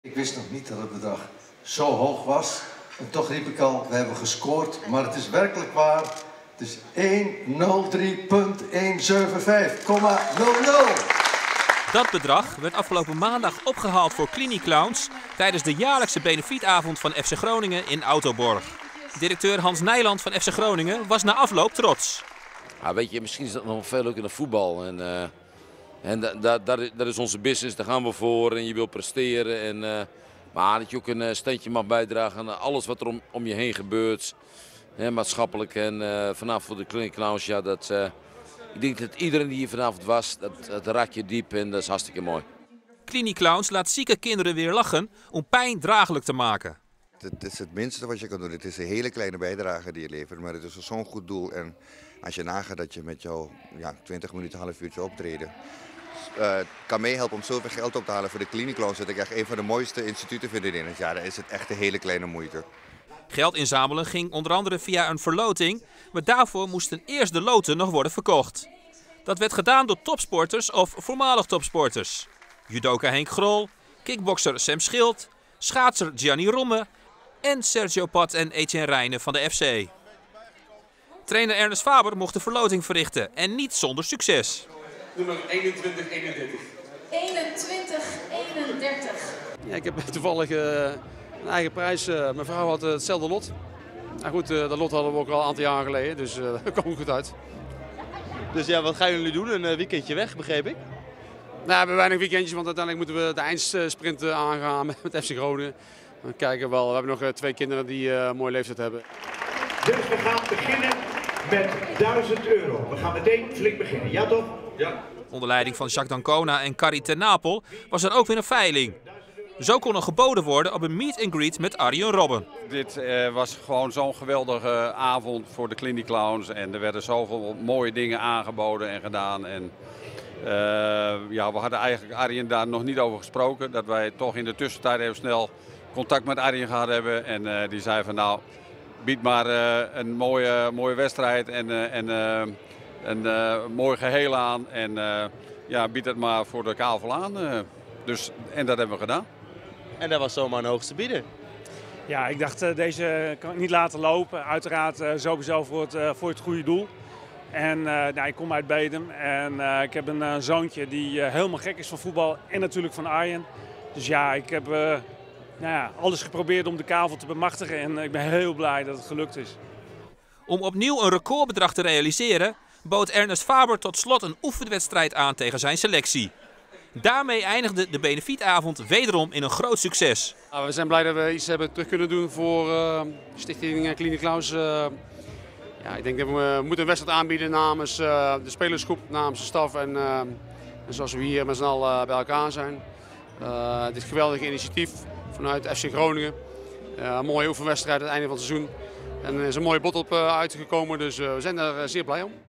Benefietavond FC Groningen voor CliniClowns
FC Groningen organiseerde 10 april 2017 een benefietavond voor CliniClowns, met een geweldige opbrengst!